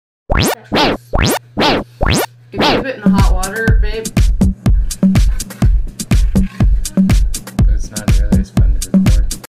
Wiggly Angel Food Cake!
A clip of us slicing a rather wiggly angel food cake. Oh, it also has some fun sound effects. ;)